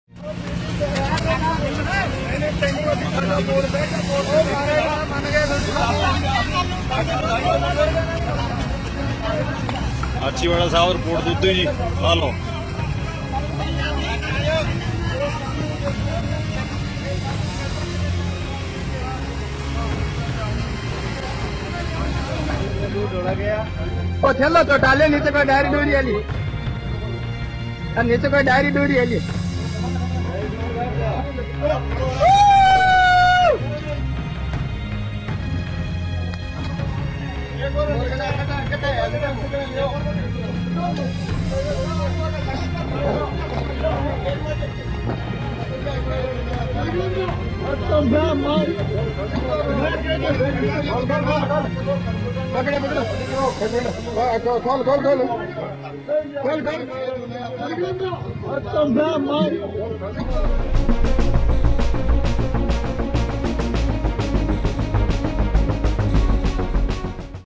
Shame on Congress Cadres - They snatched Milk from poor farmers and pour it on road.
ये कैसे किसान है जो किसी दूसरे की मेहनत मजदुरी को इस तरह जबर्दस्ती बर्बाद कर रहे है ...  दूध को जबर्दस्ती बर्बाद कर आंदोलन का नाम दे रहे है...